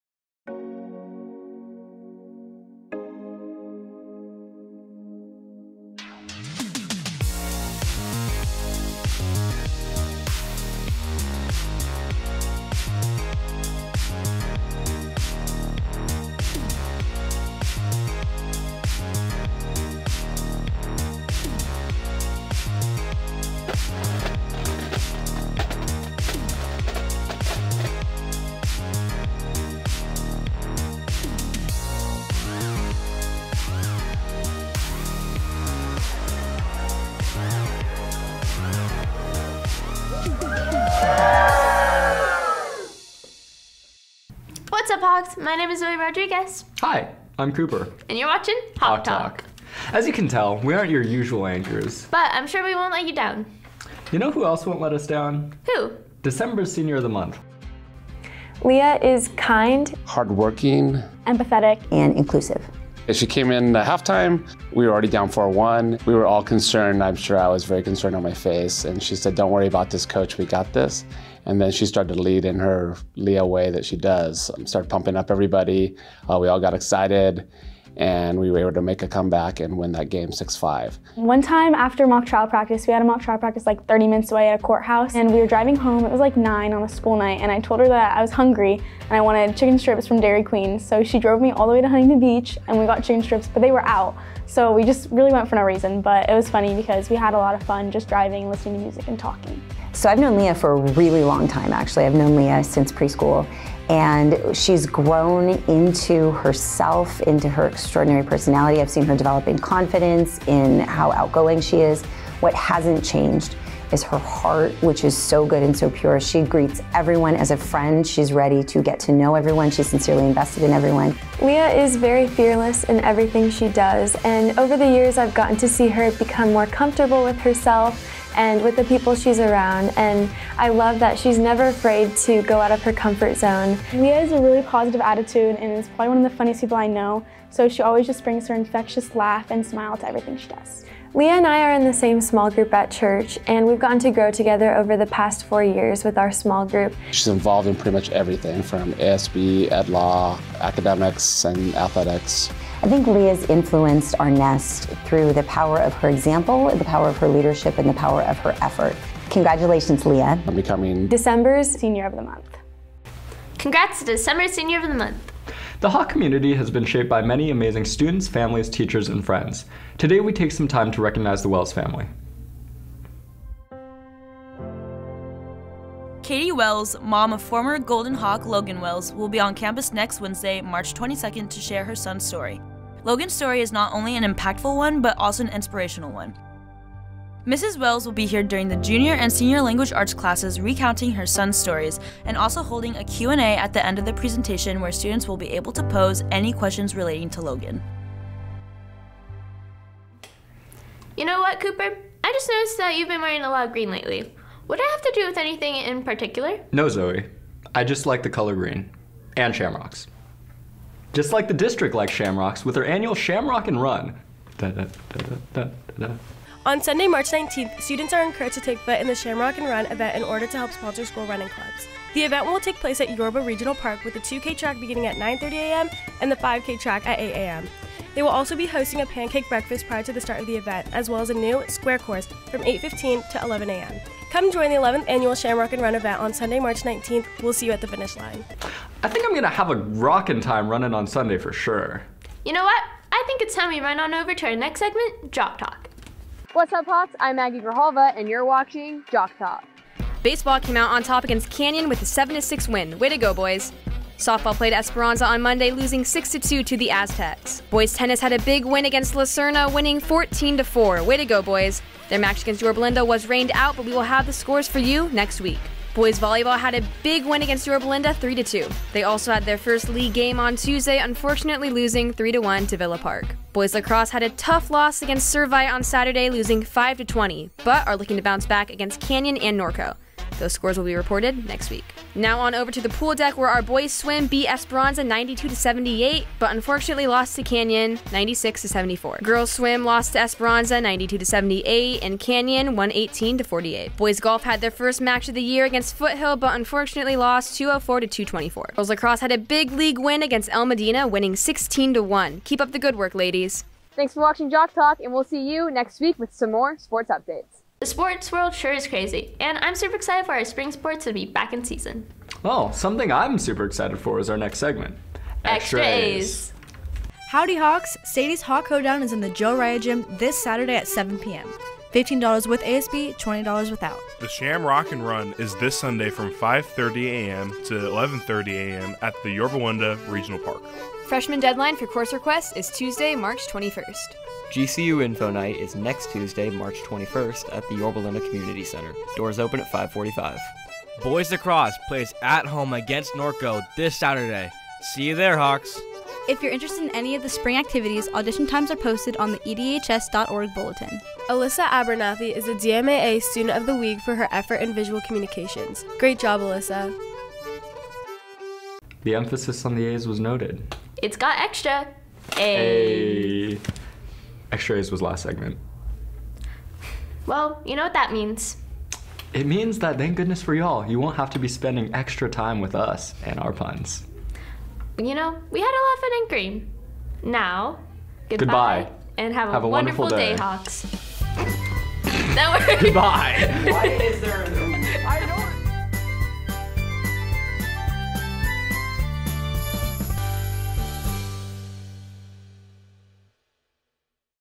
Hawk Talk 3-17-23
Hawk Talk is the award winning weekly school news show for El Dorado High School in Placentia, California.